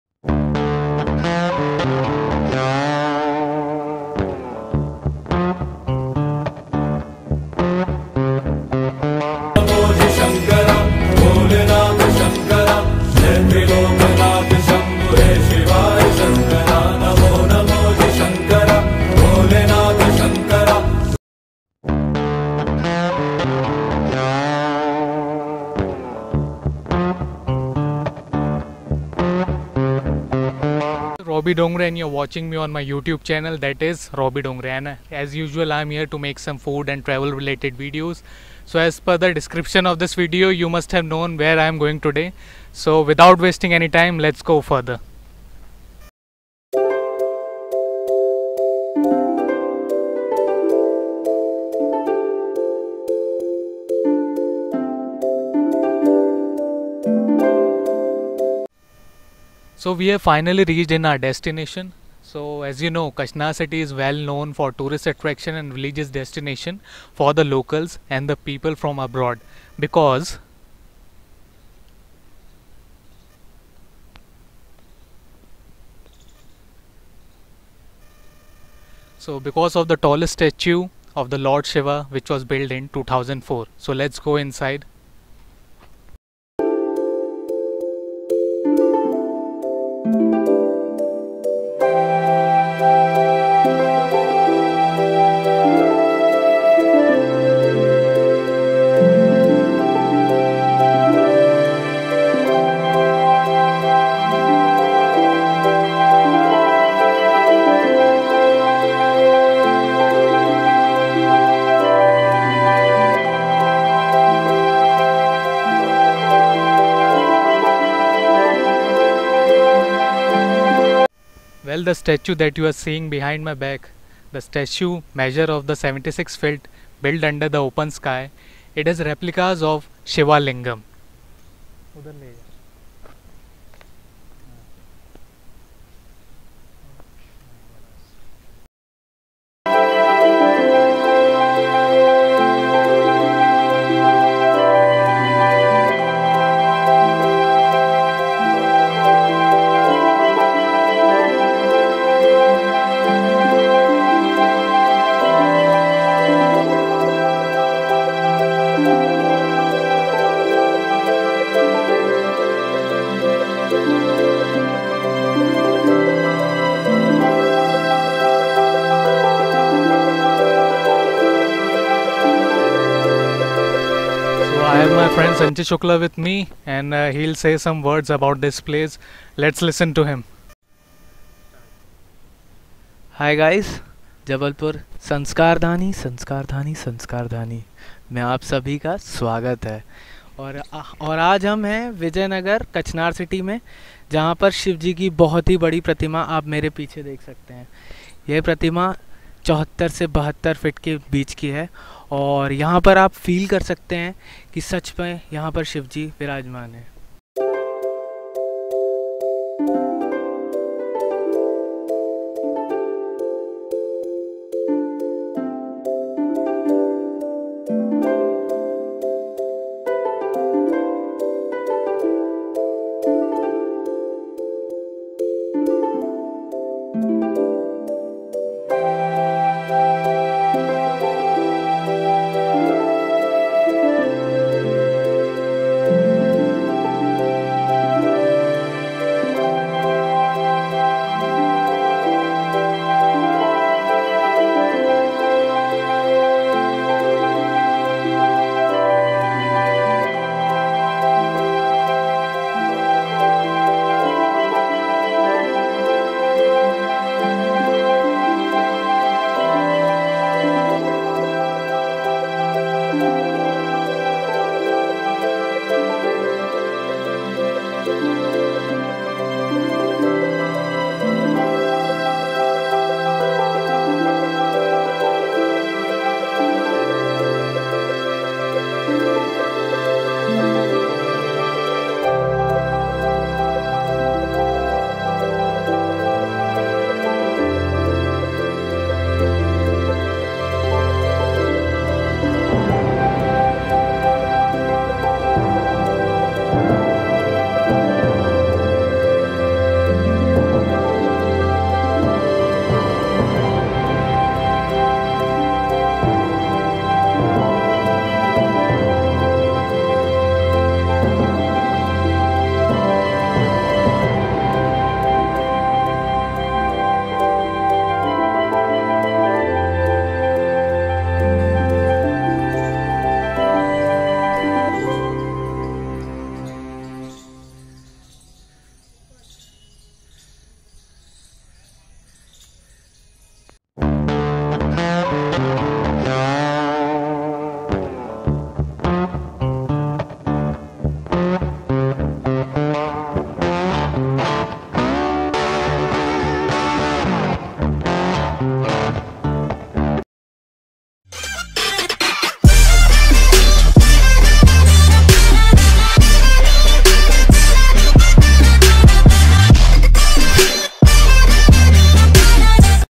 Shiva Temple Kachnar City जबलपुर के भोलेनाथ - Har Har महादेव
Lord Shiva Temple is situated in Jabalpur in Kachnar City.

The Kachnar City is well known for the tourist attraction and religious destination for the locals and the people from abroad because of the tallest statue of Lord Shiva which was built in the year 2004.

There is about 12 Jyotirlinga in the caverns on which the Lord Shiva’s idol is built. These 12 Lingam has been collected from various shrines of Lord Shiva throughout the country.